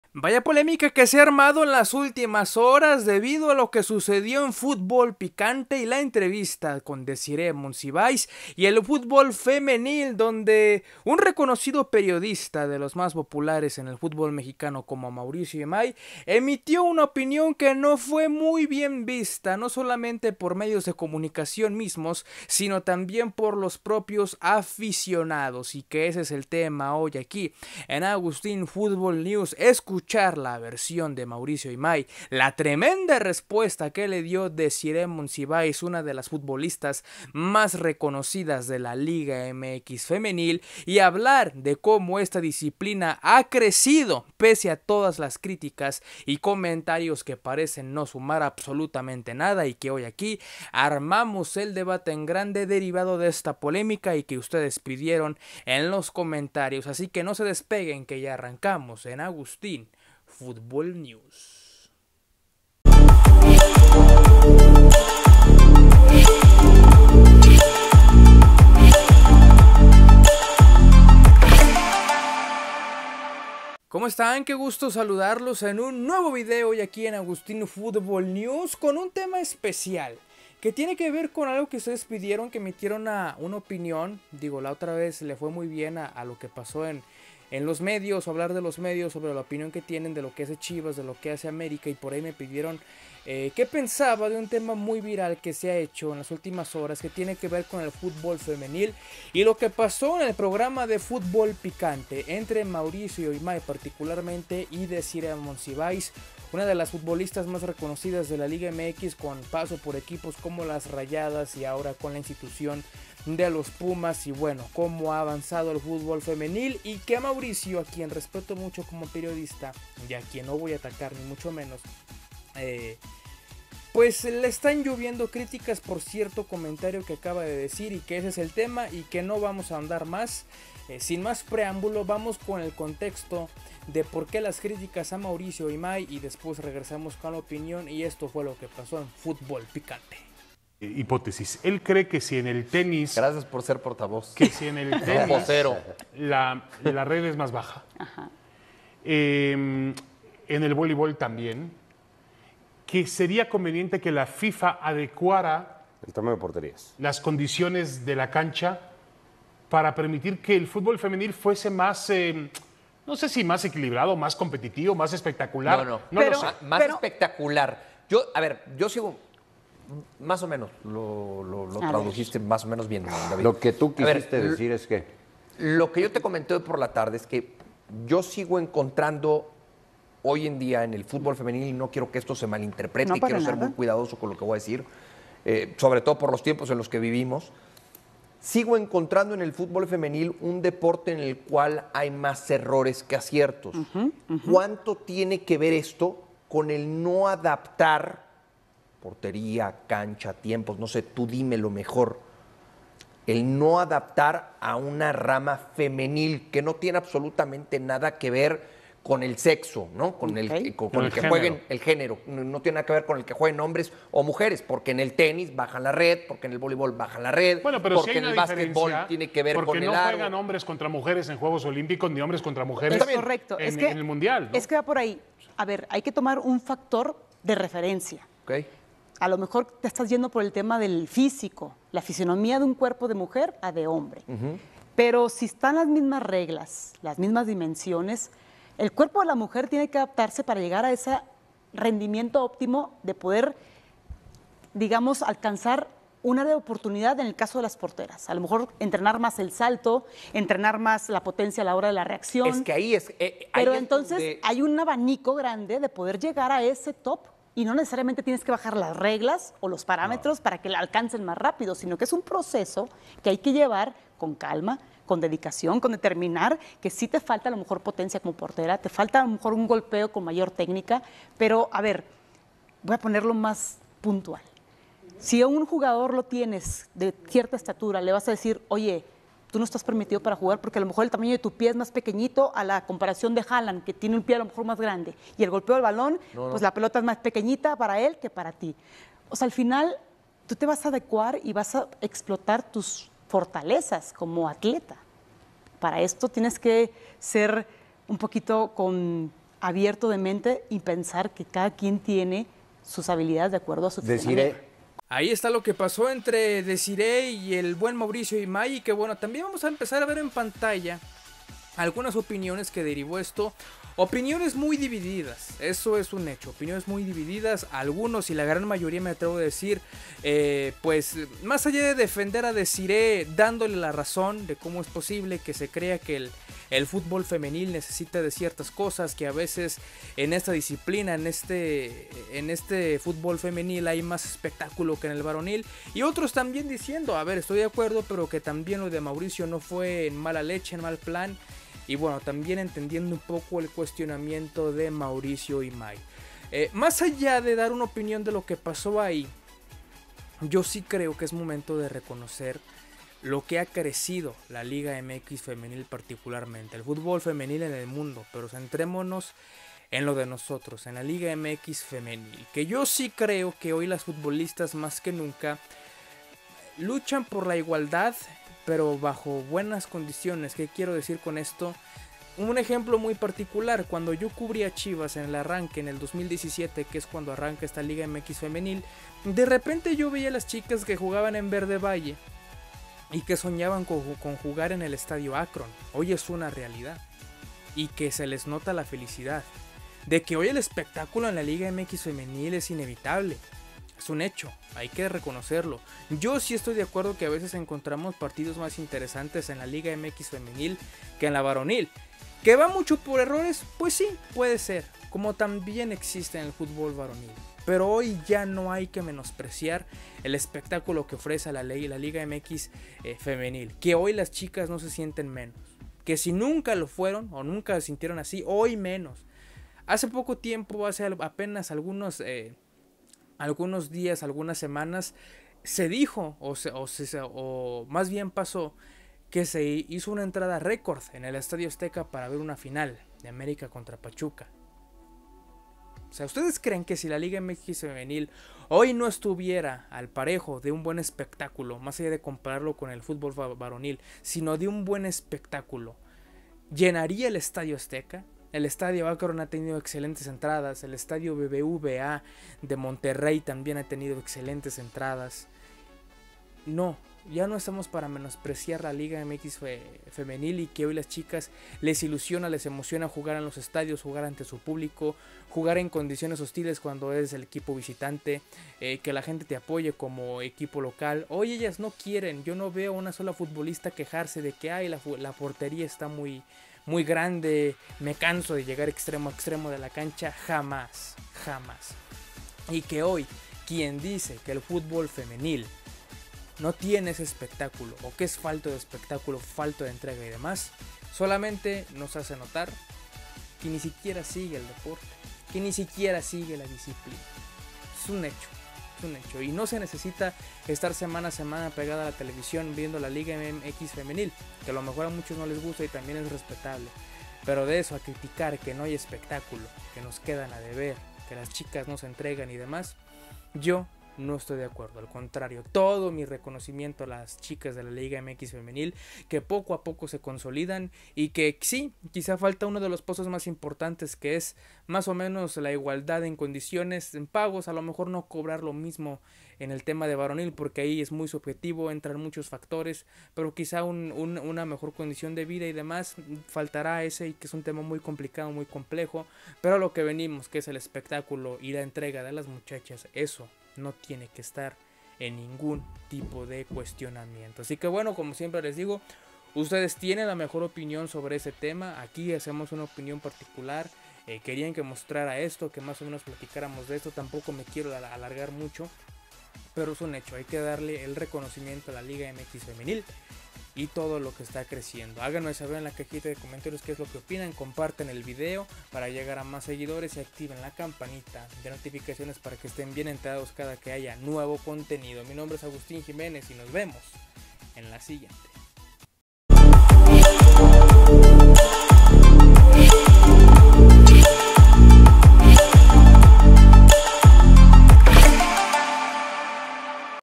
¡PERIODICAZO! EXHIBEN A MAURICIO YMAY POR PENOSO COMENTARIO AL FUTBOL FEMENIL
Mucha polémica ha causado en las últimas horas la opinión de Mauricio Ymay en la Liga MX Femenil y la popular futbolista, Desirée Monsiváis, dejó impresionados a todos con su respuesta.